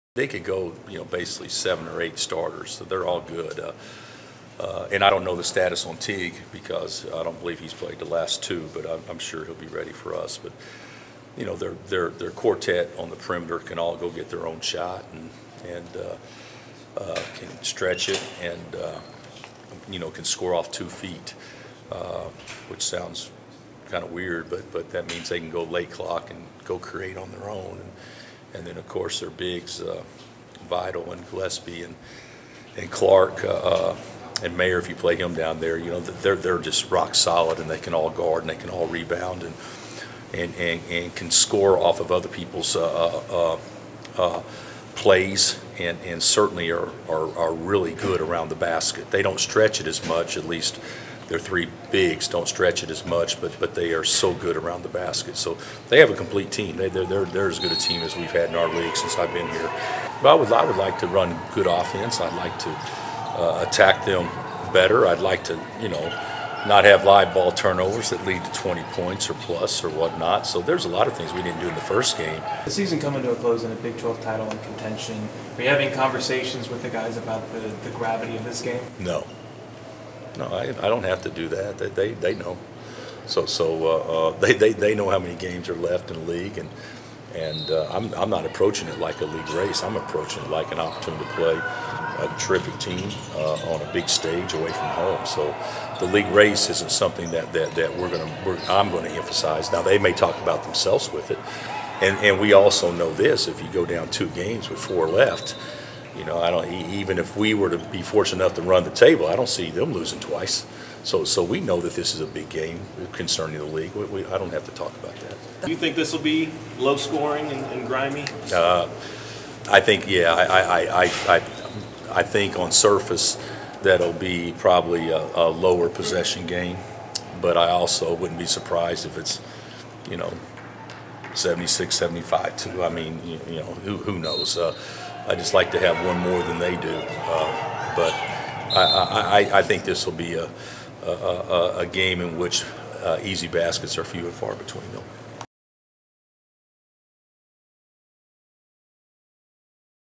Bill Self previews KU game at top-ranked Baylor
Kansas Jayhawks basketball coach Bill Self, speaking on Feb. 20, 2020, previews KU's Big 12 game against top-rated Baylor on Saturday, Feb. 22, 2020 in Waco, Texas.